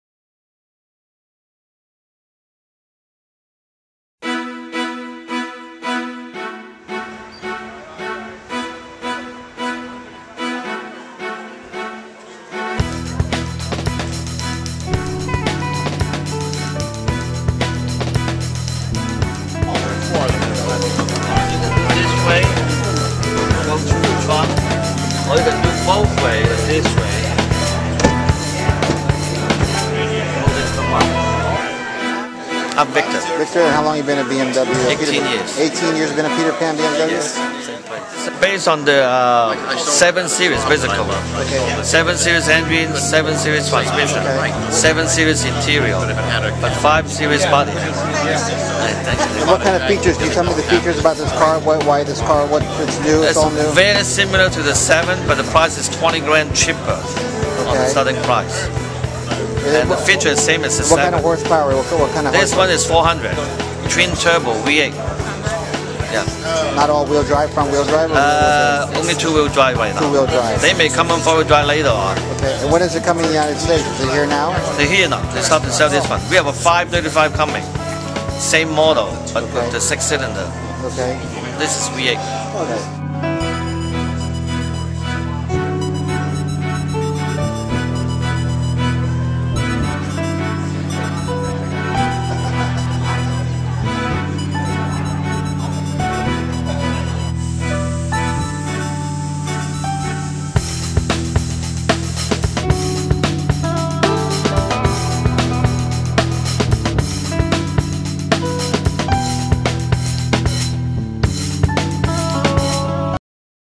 All New 2010 BMW Gran Turismo
Peter Pan BMW introduces the all new 2010 Gran Turismo recently introduced at the all new Peter Pan BMW State of the Art Service Facility located on the Peninsula. Peter Pan offers a buying and service experience second to none on how easy it is to drive a BMW. Enjoy how easy and affordable driving a New BMW really is from Peter Pan BMW.